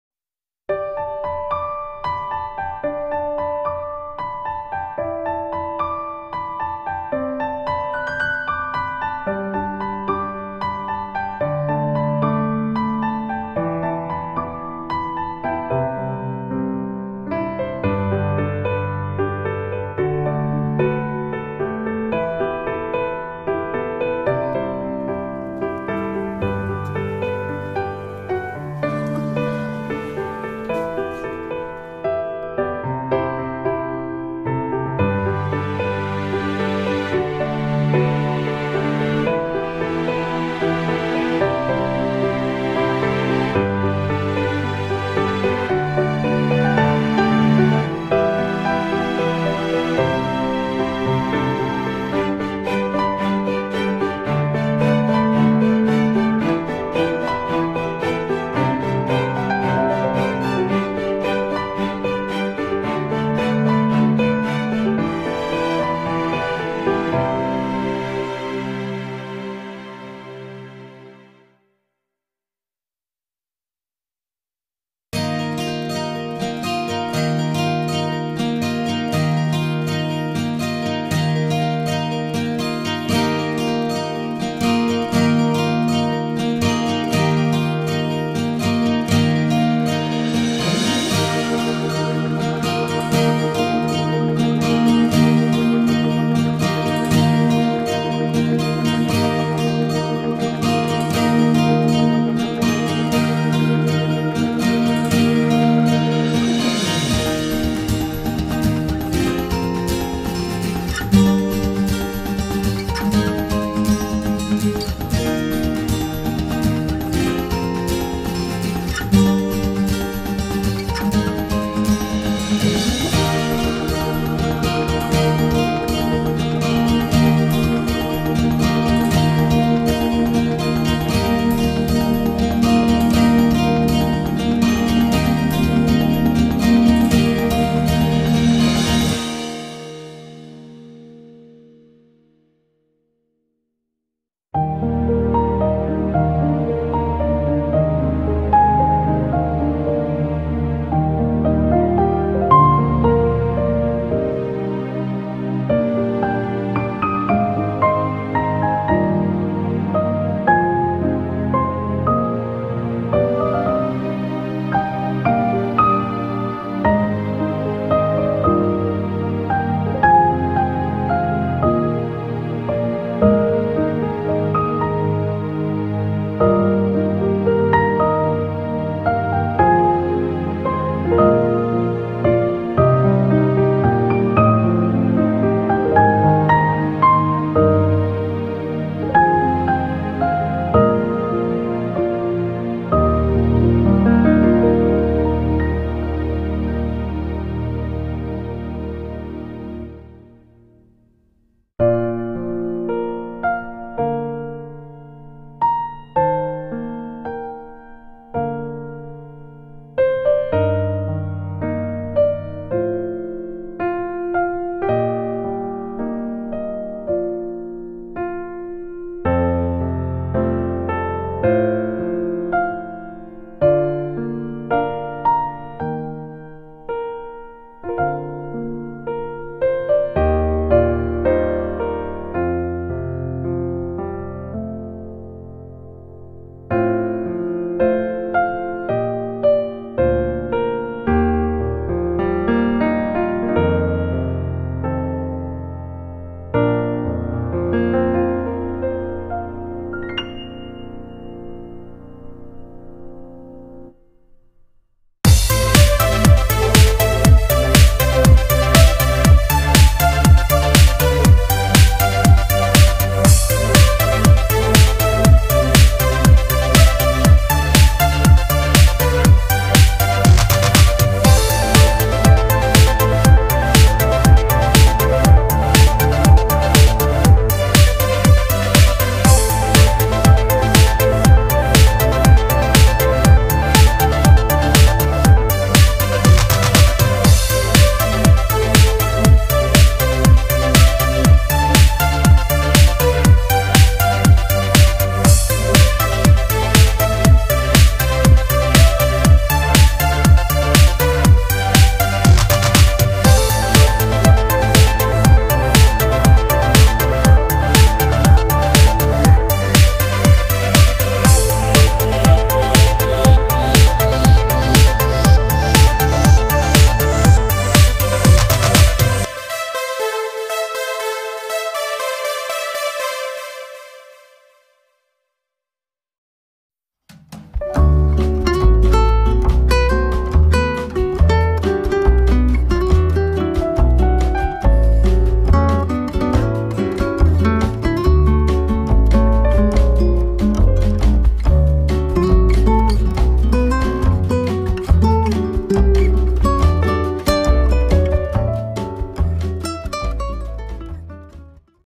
korea 김포의 공원같은 카페 글린공원 주말 사람많아 다른카페로~
korea  김포 공원같은 카페가 새로 생겼다고해서 가보는데 카페 글린공원 사람이 넘 많아요!
공원같은 카페 조용할때 다시와야할듯 빵 가격 대단!